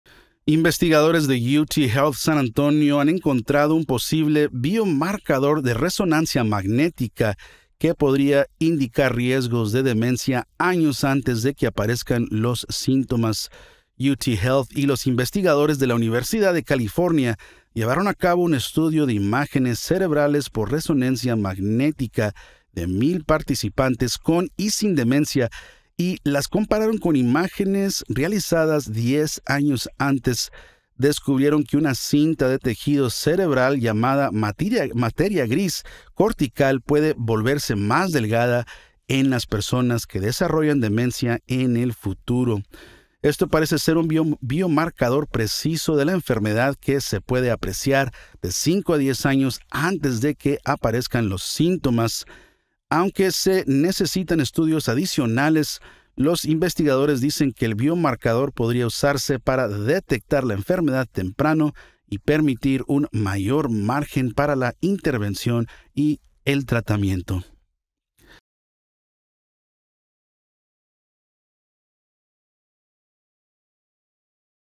UT Health SA ha encontrado un posible indicador de riesgo de demencia antes de que haya síntomas
Investigadores de UT Health San Antonio han encontrado un posible biomarcador de resonancia magnética que podría indicar riesgos de demencia años antes de que aparezcan los síntomas.